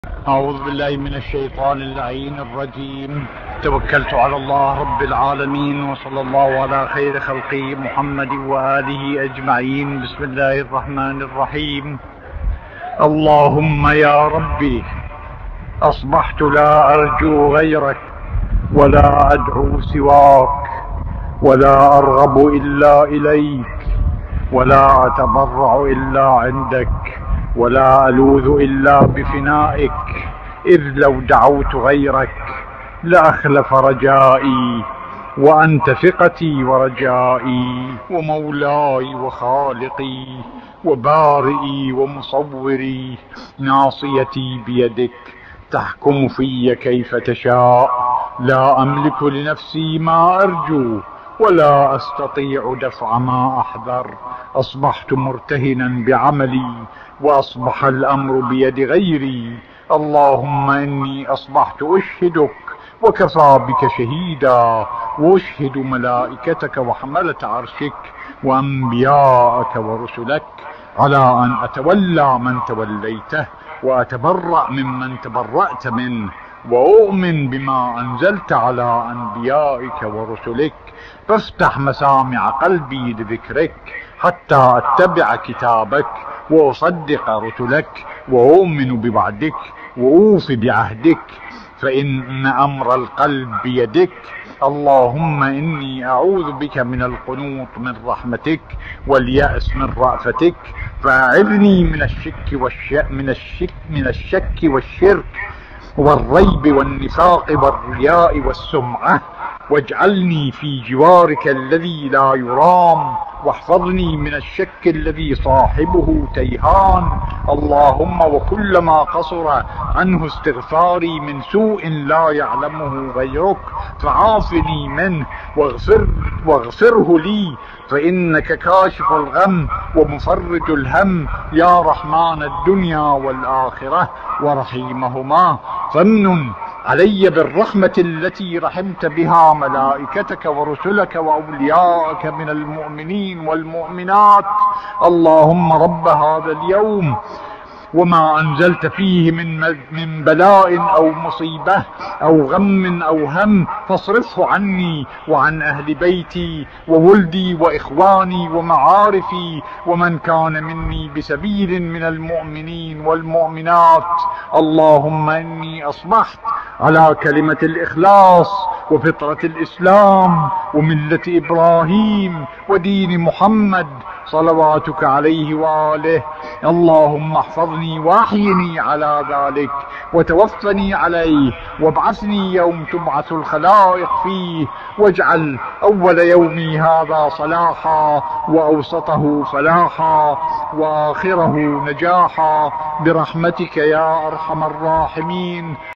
دعاء (الهي لو دعوت غيرك لاخلف رجائي) بصوت السيد الشهيد محمد الصدر (قدّس سرّه)
الوليّ المقدّس السيد الشهيد محمد_الصدر /
الجمعة #25 - الخطبة الثاني